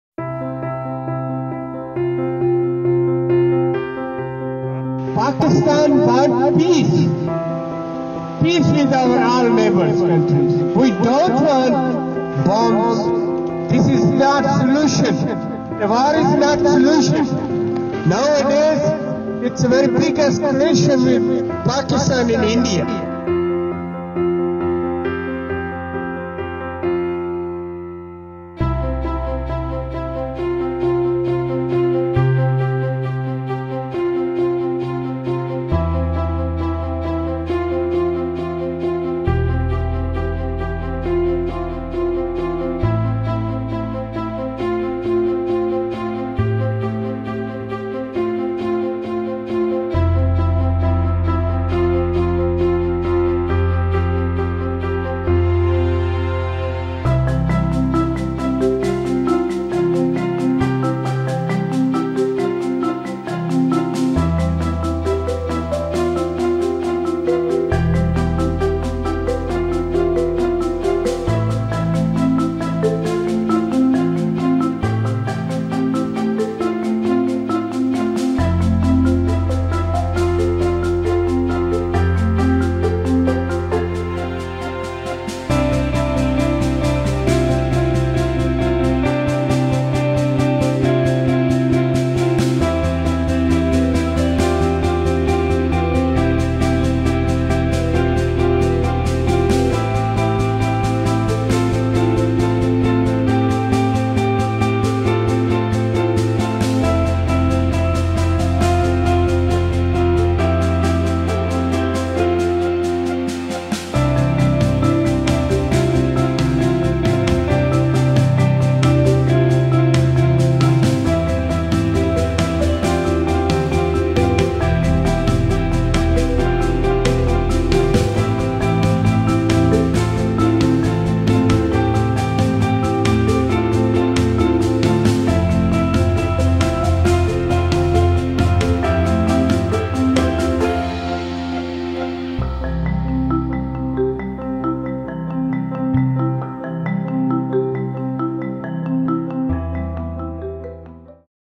Pakistan Zindabad Peace Rally | Family & Friends | All Pakistan Press Club Austria | Abdul Faheem
Abdul Faheem | Faheem.Portfolio
Video Editor, Graphic Designer, Web Designer & Dubai Reporter